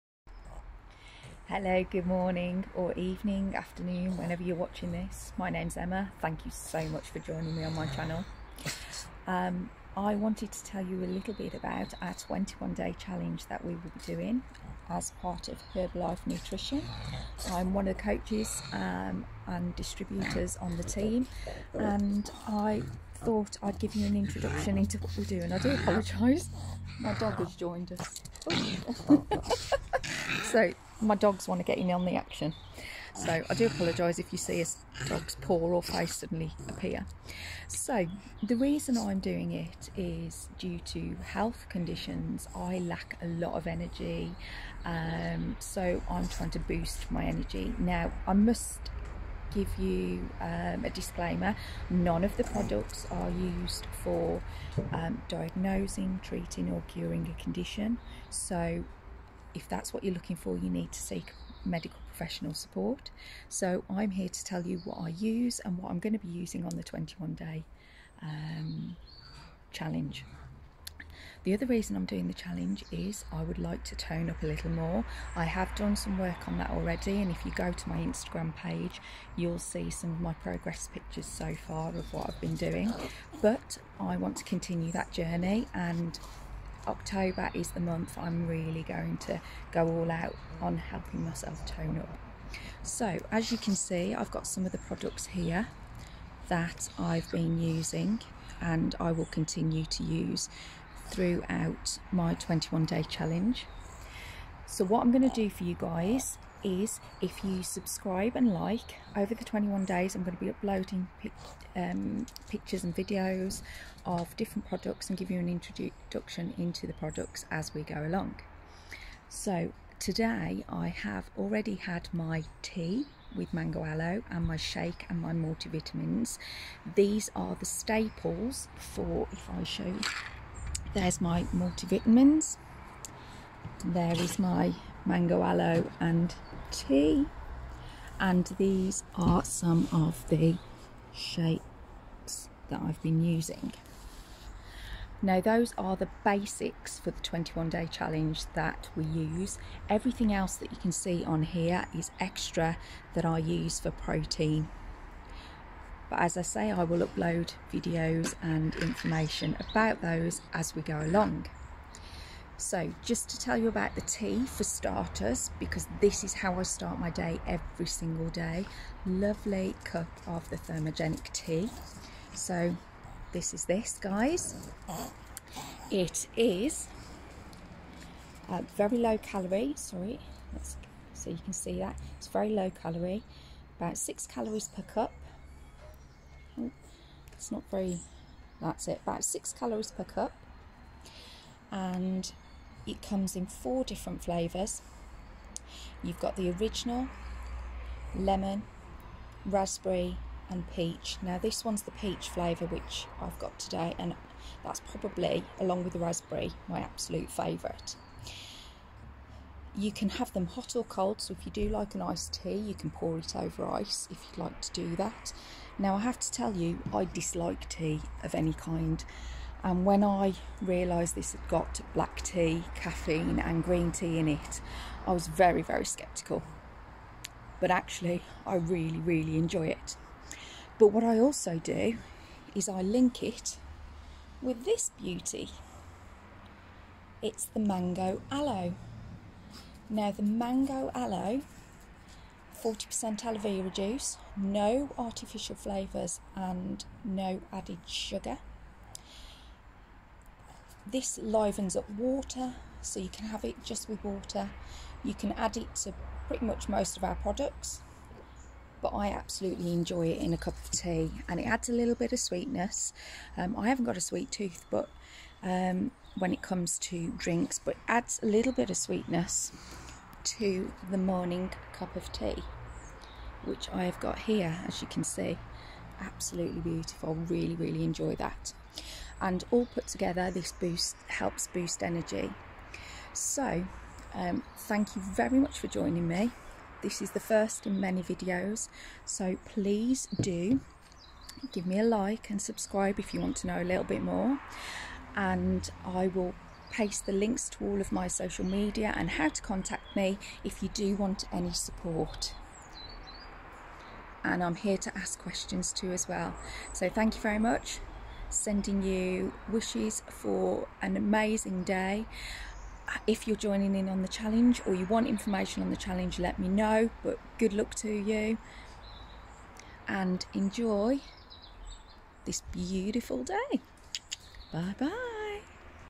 21 Day Challenge
Hello beautiful people and thank you for tuning into my channel again 😘

➡️This is day 1 of the new Herbalife 21 day challenge and an introduction to how I am increasing my energy, by having an established morning routine. ✨

➡️Over the next 21 days, I will post on a Tuesday, Thursday & Saturday to give you more information on each product I'm using and other tips of how I'm toning & continuing with 'All in October'! ✨